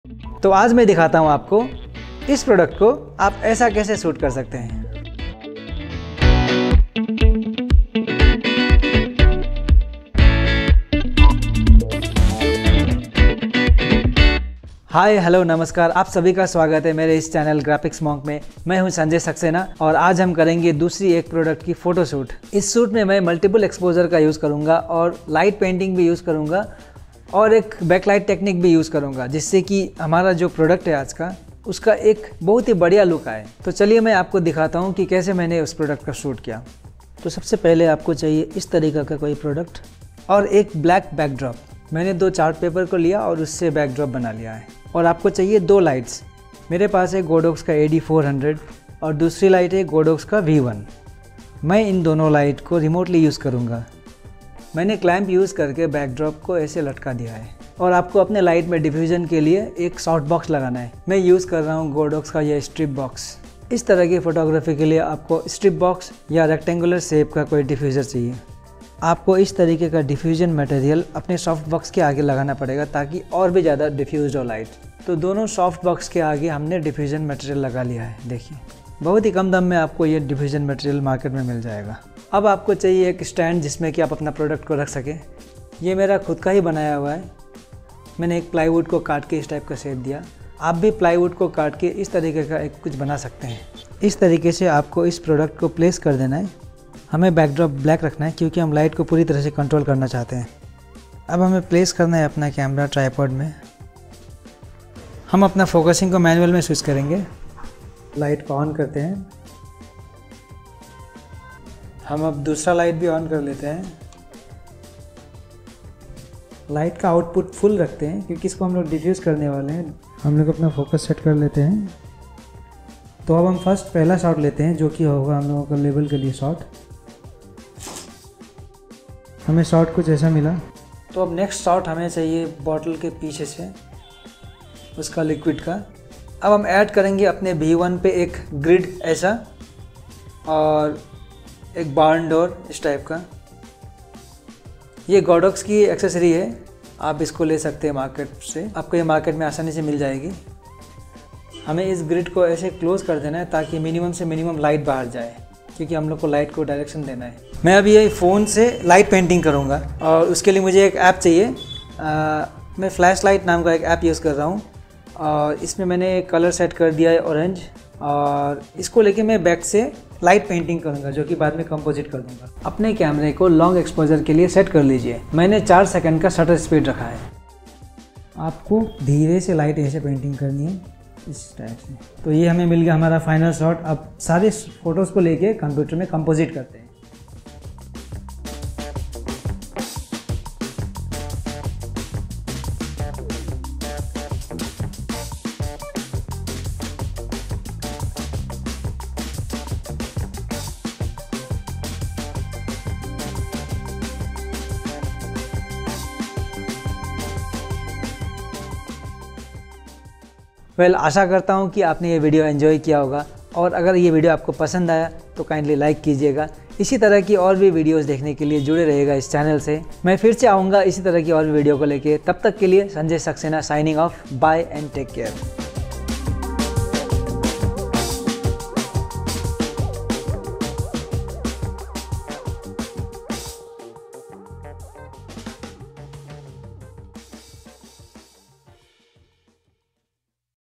Product Photography using Multiple exposure and Light painting
In this video I am shooting a product with multiple exposure, light painting and later compositing all the images for one final Photo